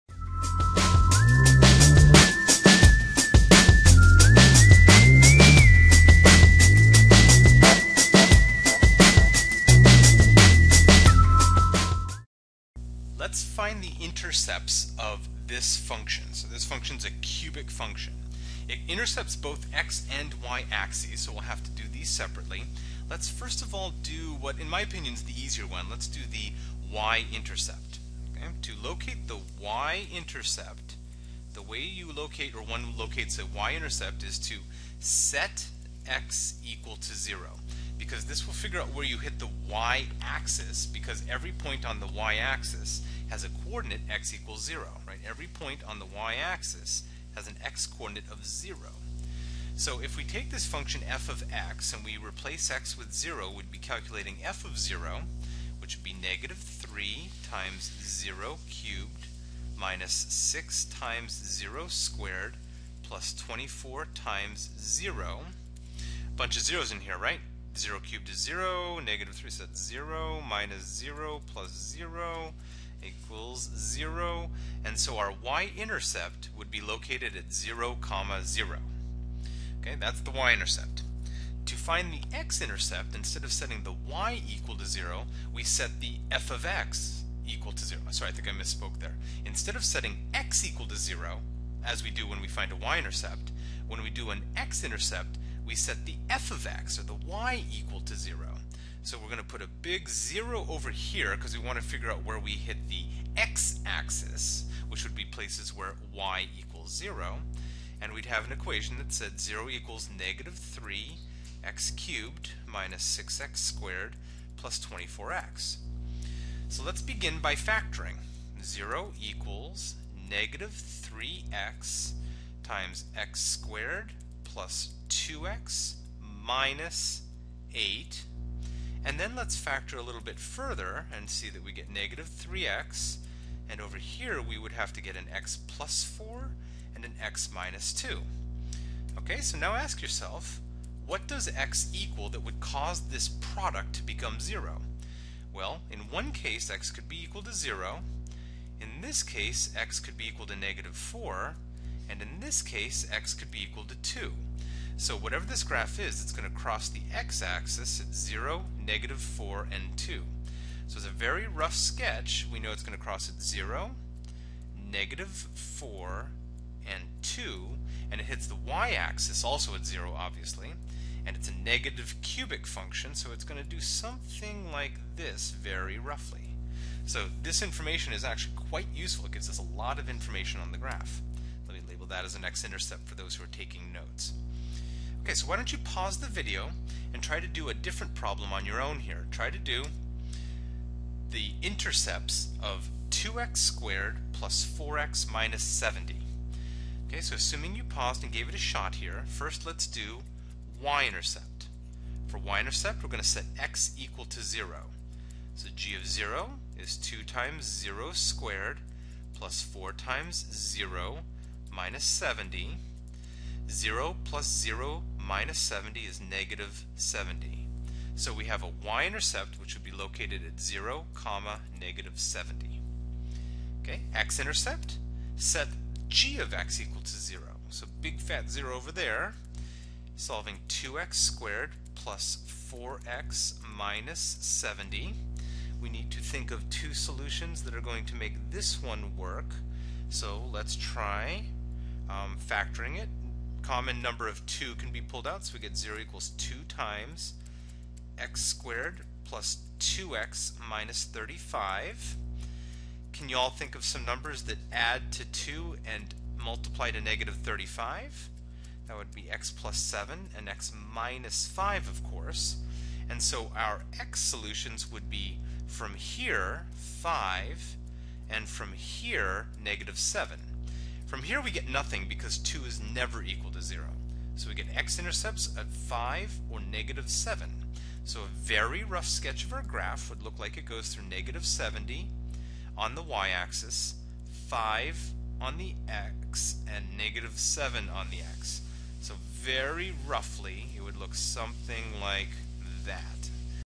Finding intercepts from the equation of a function
Finding x-  and y-intercepts given a polynomial function